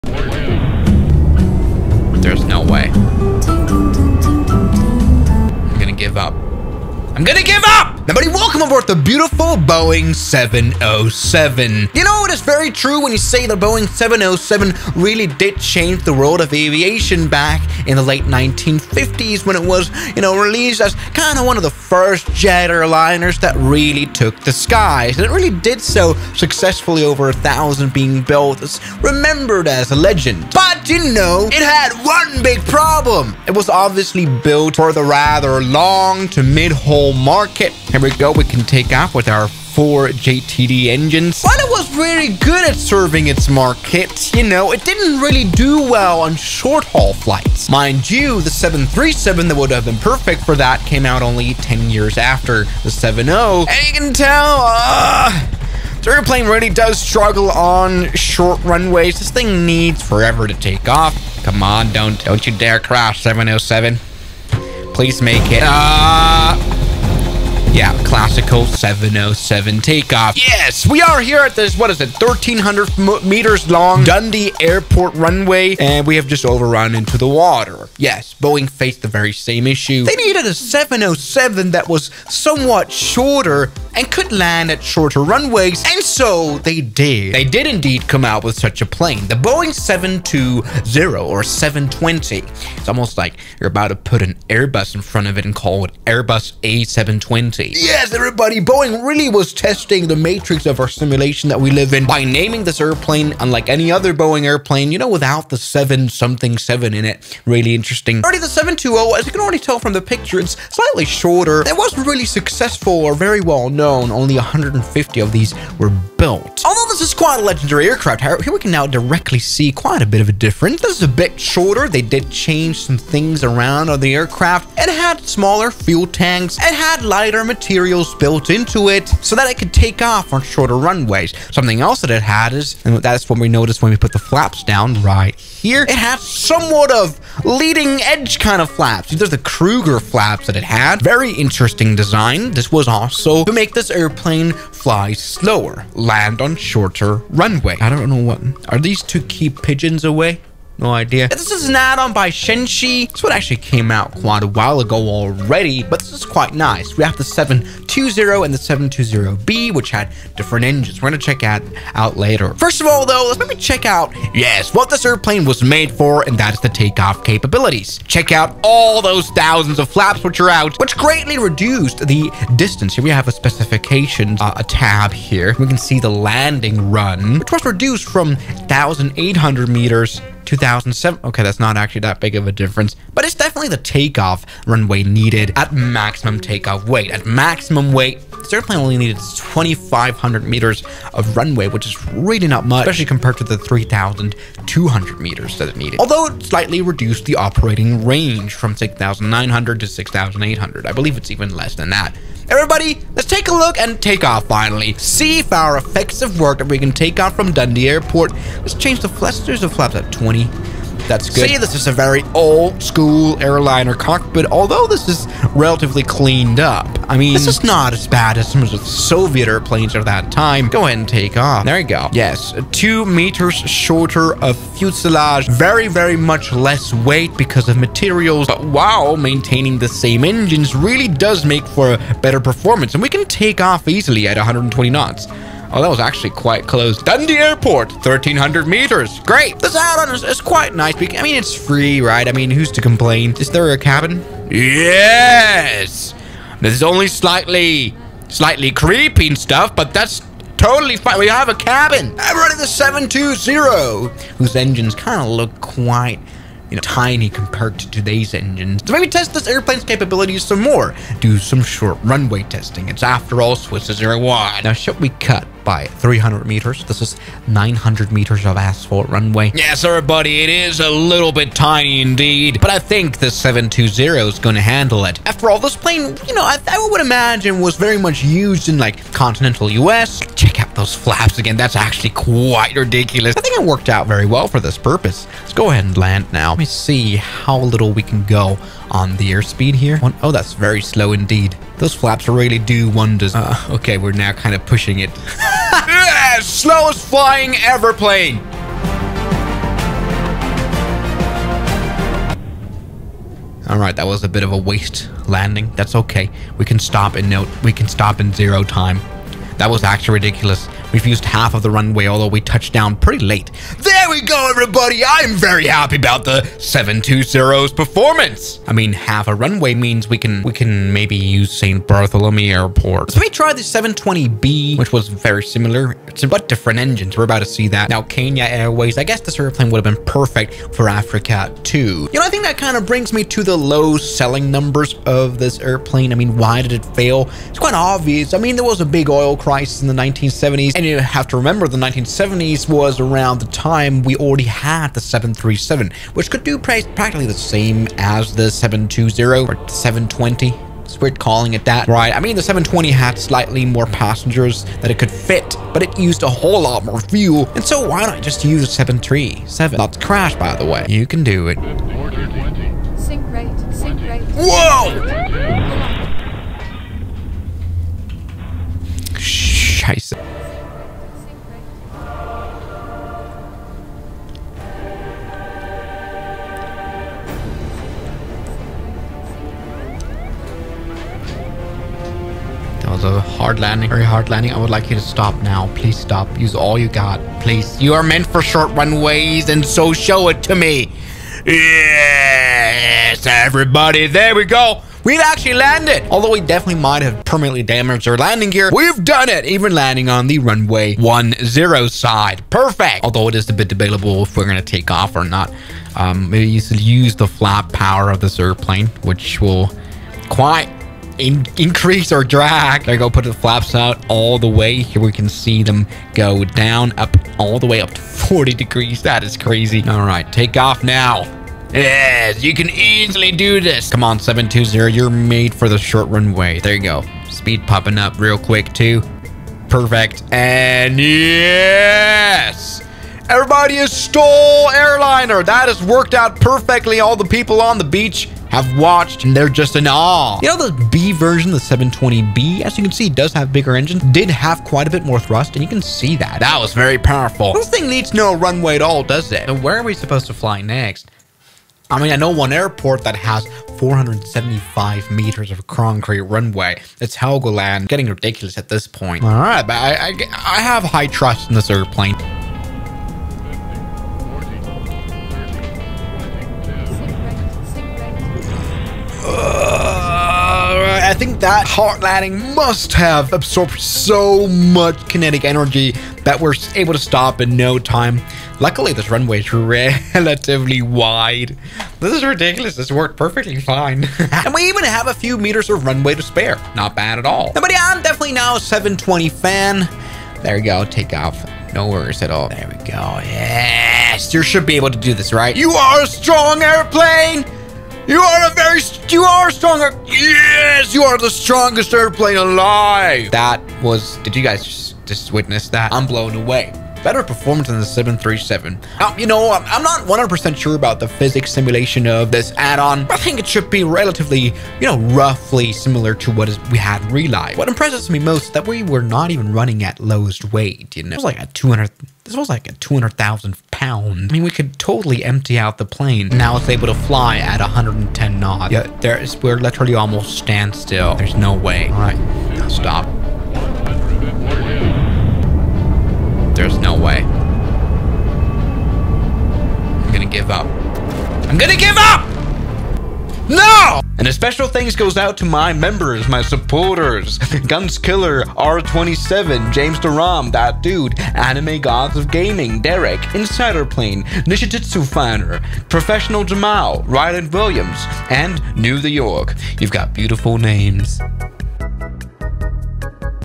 The Boeing 720 Could Land Anywhere - WHY DID IT FAIL?
Today's Video: The Boeing 720 Could Land Anywhere - WHY DID IT FAIL?
Flightsim Setup:
Airbus Joystick
Boeing Yoke

Graphics Card

PC:
16" MacBookPro for editing.

©Swiss001 2021